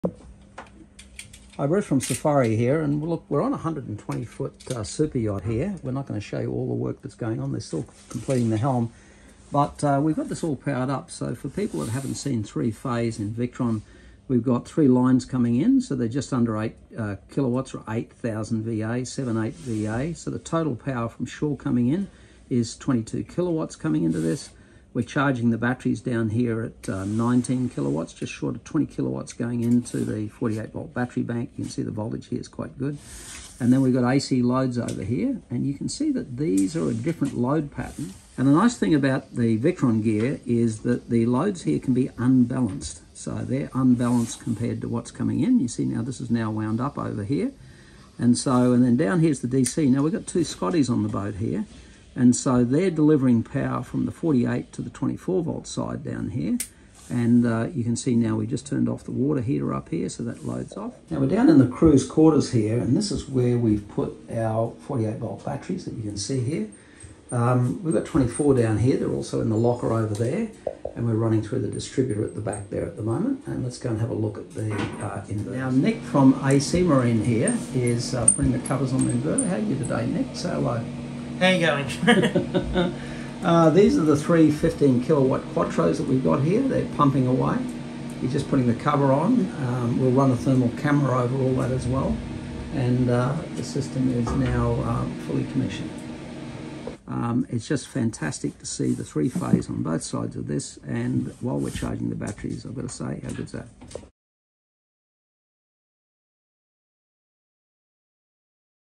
120Ft Superyacht Electrical Refit 45kW 3Phase Power 48V 60,000Wh Lithium with 2 x Scotty 24-48 6000W
120Ft Superyacht Electrical Refit 45kW 3Phase Power 48V 60,000Wh Lithium
Just a sneak preview of the new power generation system for this 120Ft Fiedship . Its a refit managed by Steve and the team at Proyacht located in Boatworks Coomera. The power system is setup with 3 Victron Quattro 15kw per phase charging 60,000Wh of Lithium at 48V. That will allow Johnny, the ship’s owner, to operate for over a day with no genset running. Now the “old school” will prefer to just run the genset. But the new age owner with coffee and toast on the stern deck wants peace and quiet early morning. Silence is a luxury on a boat. A silent night connects you with nature. Its a case of “less is more”. Installation by Harald & Nick at ASea Marine including the complex 3 phase switching on the 125A shore power bow line or the 63A stern line. We will do a full walk through once its in the water showcasing the 2 x 24-48V Scottys which at 6kw are transferring power to the 24V starter battery’s and 24V power system.